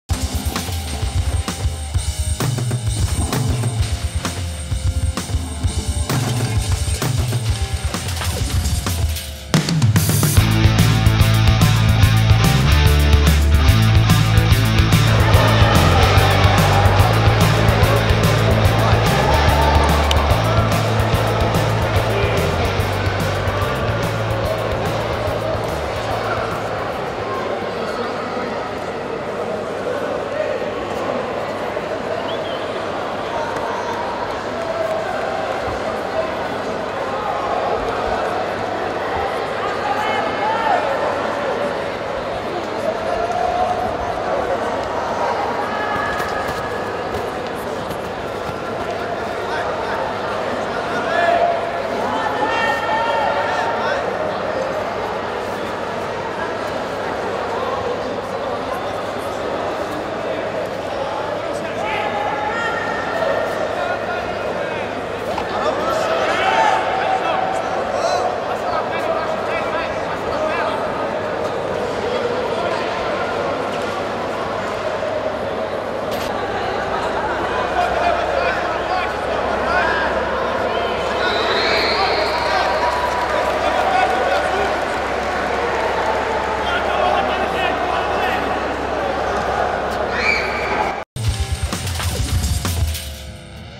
MUNDIAL CBLP 2019 - Nilton Sergio x Jorge Gomes
CAMPEONATO MUNDIAL BJJ PRO - CBLP
31 de agosto & 01 de setembro de 2019
Ginásio Paulo Sarasate - Fortaleza - Ceará